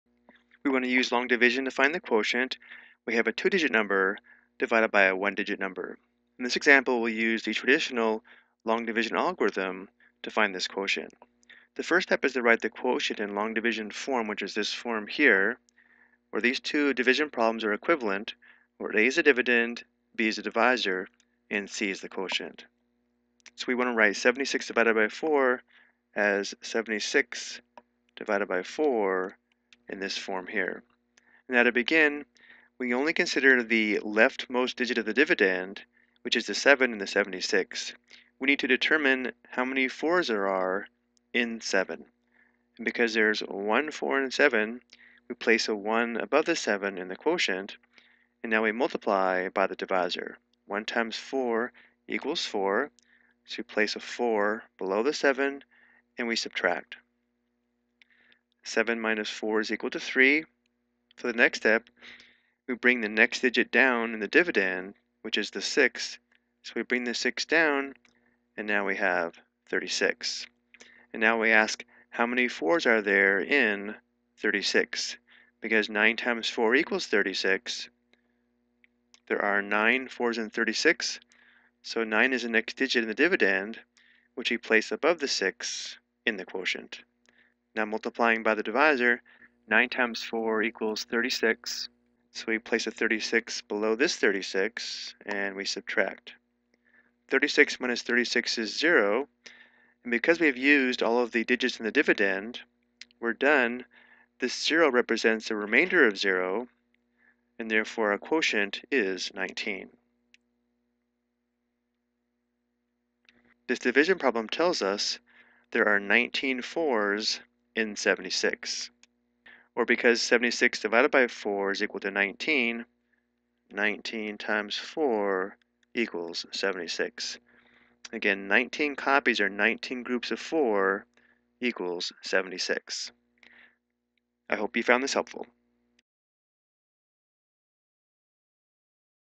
Ex: Long Division - Two Digit Divided by One Digit (No Remainder)
This video explains how to perform division using the tradition long division algorithm.